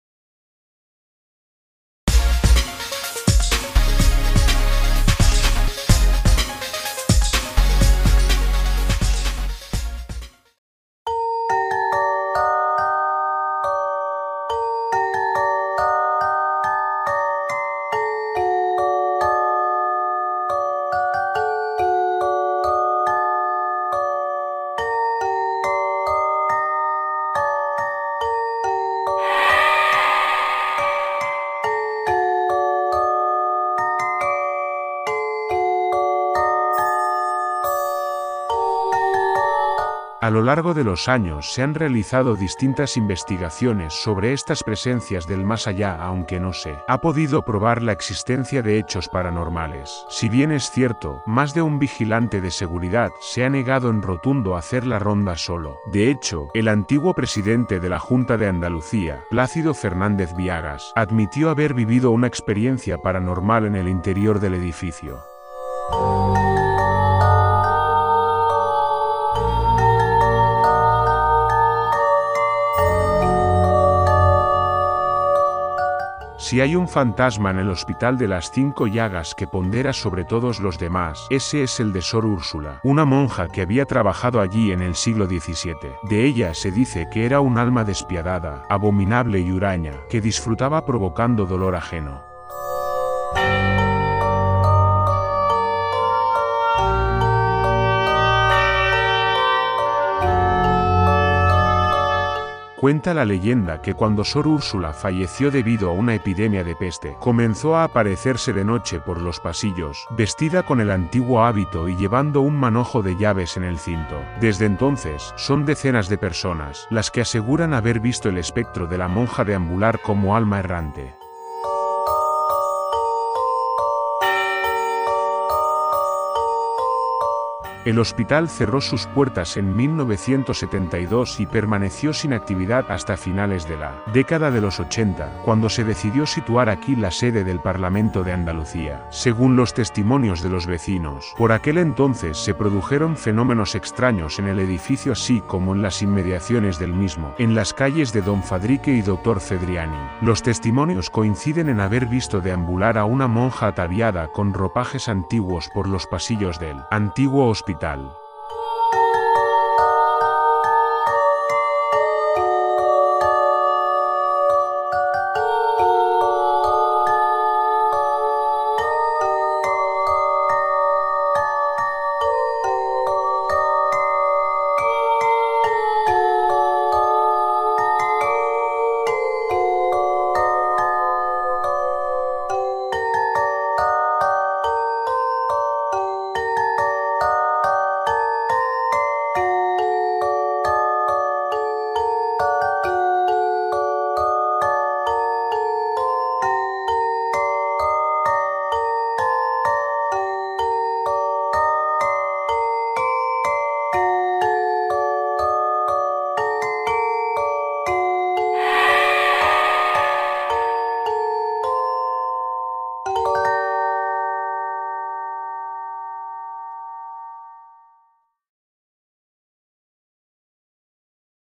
El Fantasma del Hospital de las Cinco Llagas Parlamento de Andalucía
A lo largo de los años se han realizado distintas investigaciones sobre estas presencias del más allá aunque no se ha podido probar la existencia de hechos paranormales. Si bien es cierto, más de un vigilante de seguridad se ha negado en rotundo a hacer la ronda solo. De hecho, el antiguo presidente de la Junta de Andalucía, Plácido Fernández Viagas, admitió haber vivido una experiencia paranormal en el interior del edificio.

Si hay un fantasma en el Hospital de las Cinco Llagas que pondera sobre todos los demás, ese es el de Sor Úrsula, una monja que había trabajado allí en el siglo XVII. De ella se dice que era un alma despiadada, abominable y huraña, que disfrutaba provocando dolor ajeno.

Cuenta la leyenda que cuando Sor Úrsula falleció debido a una epidemia de peste, comenzó a aparecerse de noche por los pasillos, vestida con el antiguo hábito y llevando un manojo de llaves en el cinto. Desde entonces, son decenas de personas, las que aseguran haber visto el espectro de la monja deambular como alma errante.

El Hospital cerró sus puertas en 1972 y permaneció sin actividad hasta finales de la década de los 80, cuando se decidió situar aquí la sede del Parlamento de Andalucía. Según los testimonios de los vecinos, por aquel entonces se produjeron fenómenos extraños en el edificio así como en las inmediaciones del mismo, en las calles de Don Fadrique y Doctor Fedriani. Los testimonios coinciden en haber visto deambular a una monja ataviada con ropajes antiguos por los pasillos del antiguo hospital .